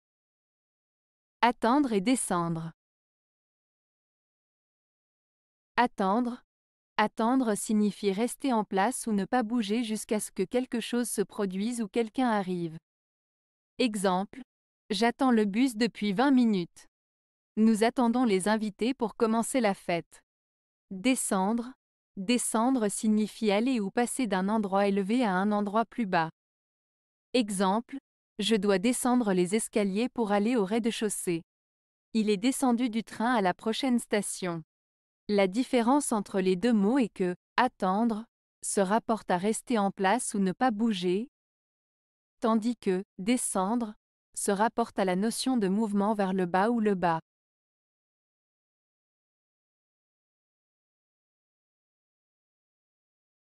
Attendre et Descendre (Paronymes)
Attendre:
Attendre signifie rester en place ou ne pas bouger jusqu'à ce que quelque chose se produise ou quelqu'un arrive.

Exemples:
- J'attends le bus depuis vingt minutes.
- Nous attendons les invités pour commencer la fête.

Descendre:
Descendre signifie aller ou passer d'un endroit élevé à un endroit plus bas.

Exemples:
- Je dois descendre les escaliers pour aller au rez-de-chaussée.
- Il est descendu du train à la prochaine station.

Différence entre les 2 mots:

La différence entre les deux mots est que "attendre" se rapporte à rester en place ou ne pas bouger, tandis que "descendre" se rapporte à la notion de mouvement vers le bas ou le bas.